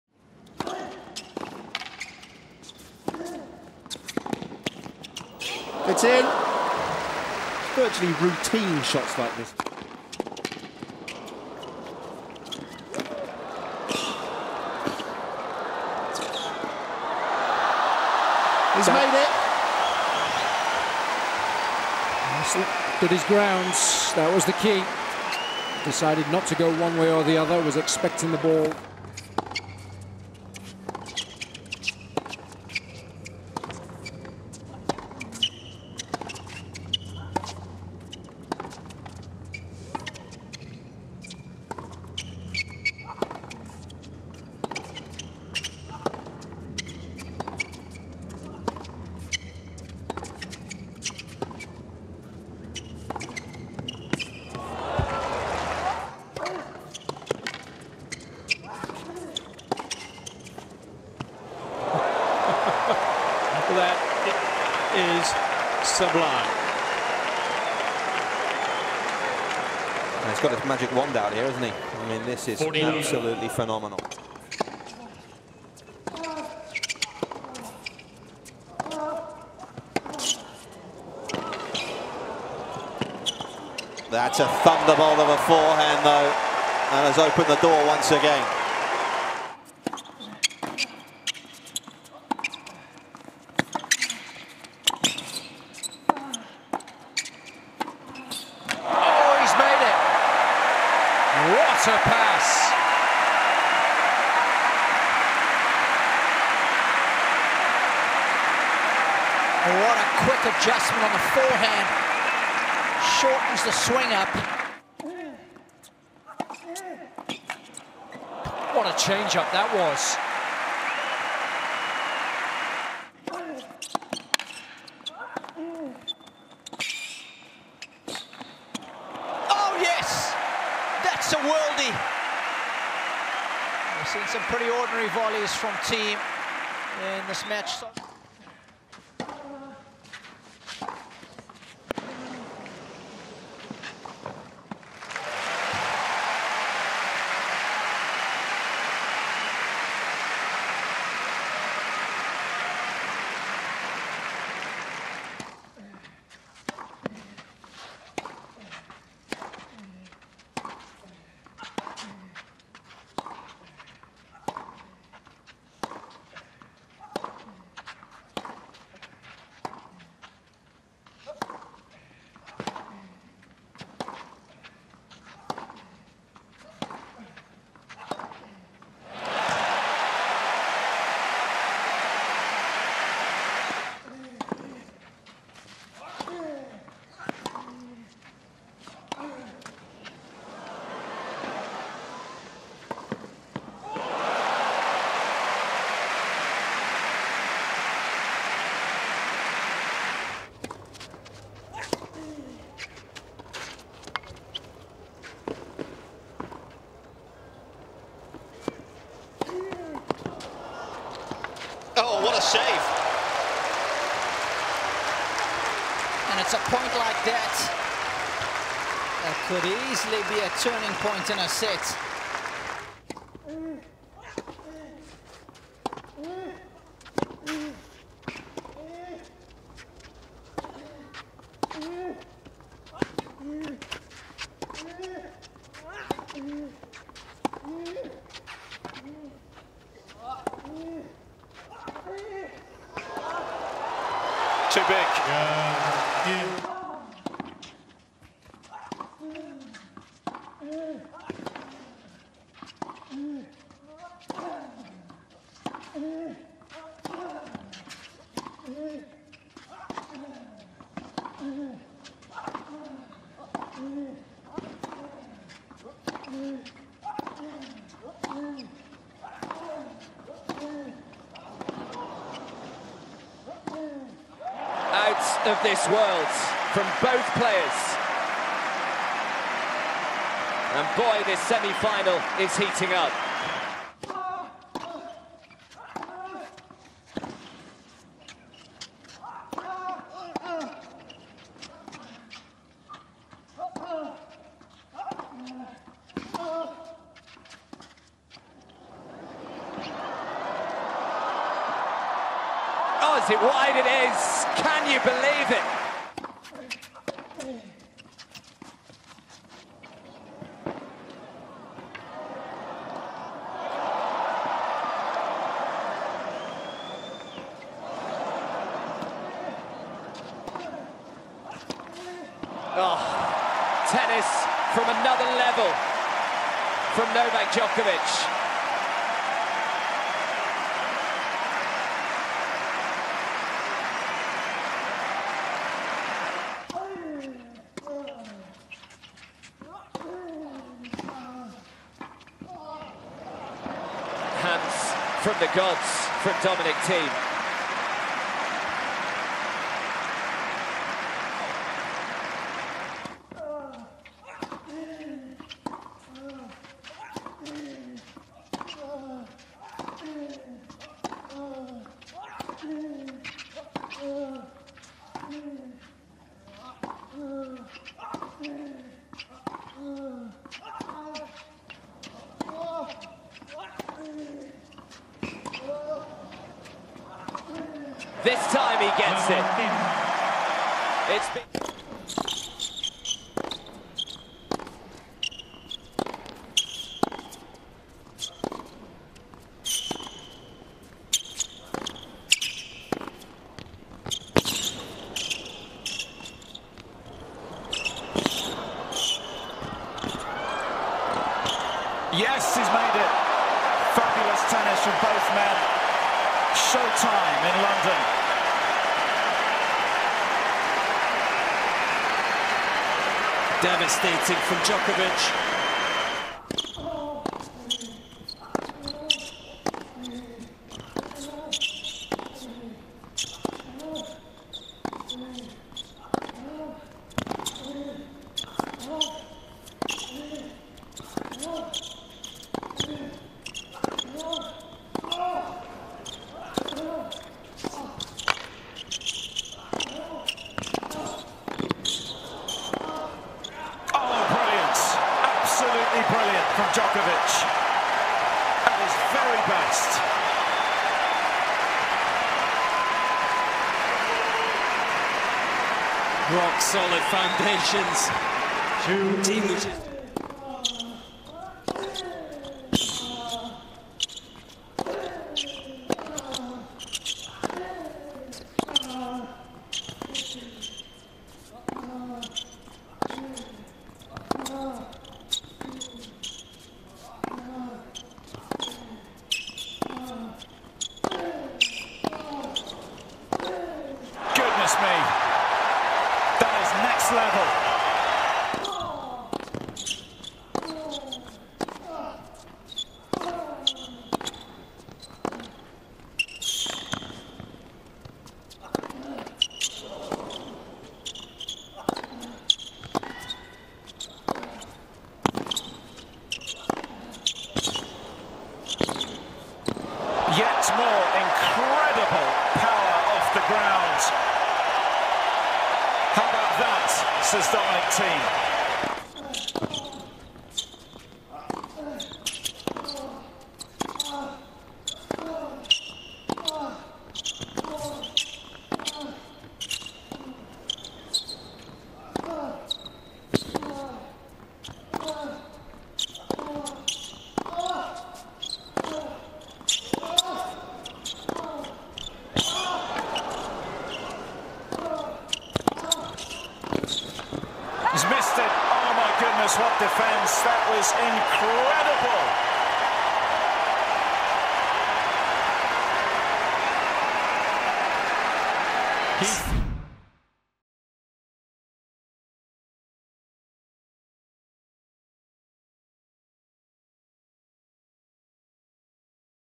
Novak Djokovic vs Dominic Thiem: Best ATP Shots & Rallies!
Relentless rallies and stunning shot-making. These two can do it all! Watch official ATP tennis streams from every tournament